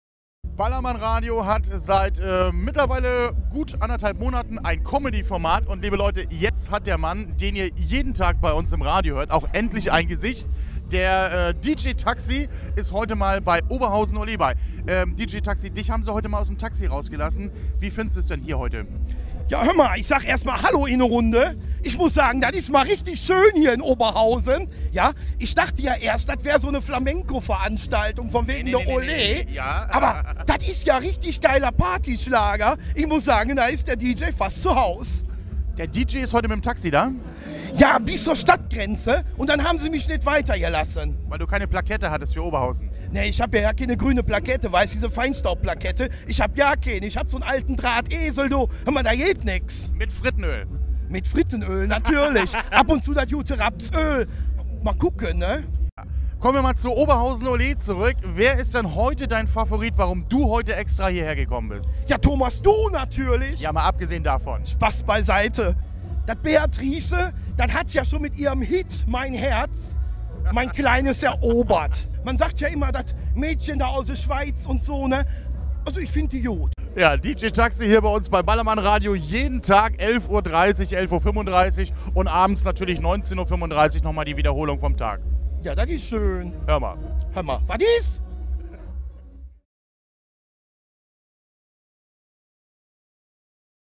Ballermann Radio on Tour Oberhausen Ole 2013 - HÖMMA
Zu Gast unser DJ Taxi, hören kann man ihn von Mo - Fr um 11:35 und 19:35 Uhr. Mit seinen Klatsch und Tratsch Geschichten.
Ballermann Radio
„Ballermann®" ist eine geschützte Marke der A.Engelhardt Markenkonzepte GmbH